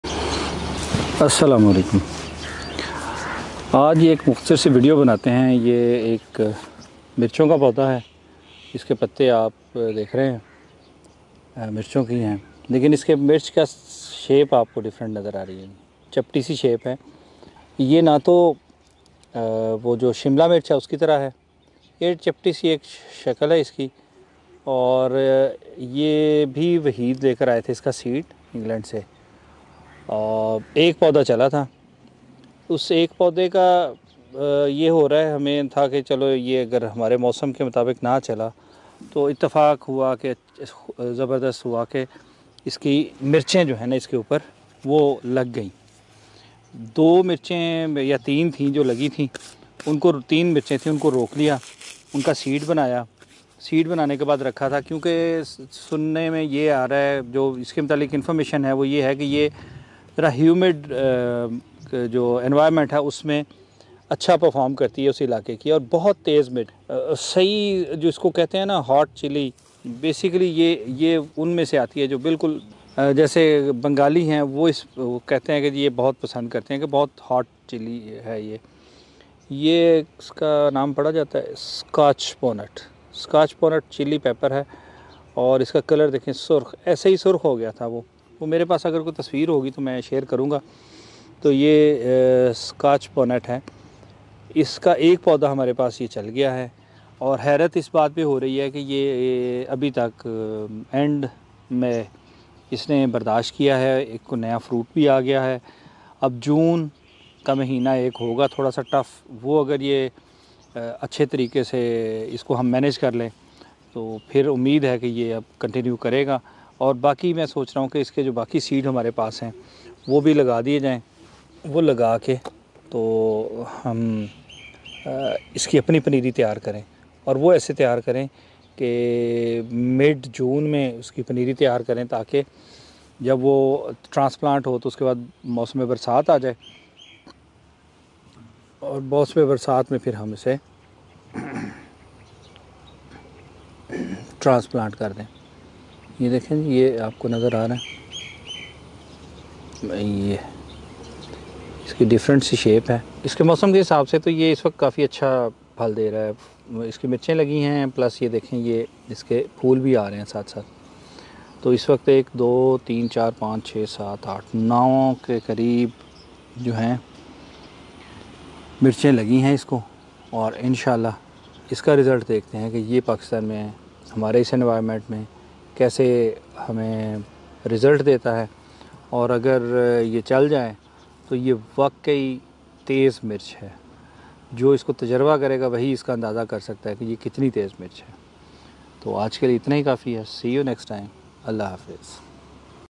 Scotch bonnet in Pakistan|سب سے تیز مرچ سکاچ بونٹ کا پاکستان میں تجربہ
Scotch bonnet in Pakistan|
سب سے تیز مرچ سکاچ بونٹ کا پاکستان میں تجربہ